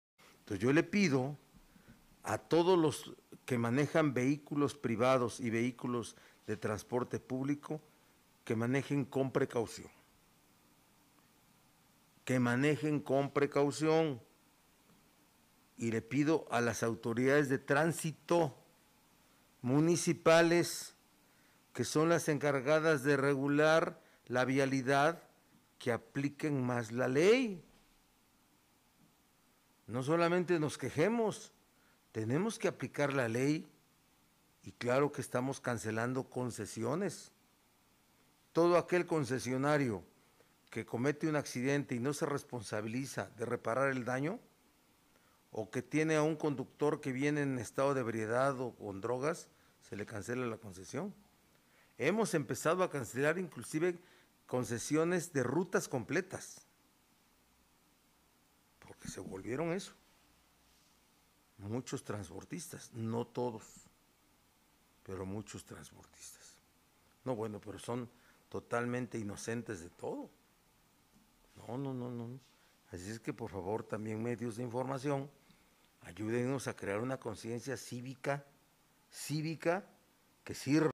Miguel Barbosa exhortó a las direcciones de Tránsito municipales a regular la normativa vial
El gobernador de Puebla, Miguel Barbosa exhortó a las direcciones de Tránsito municipales a regular la normativa vial a transportistas para que no sean omisos ante la provocación de un accidente vial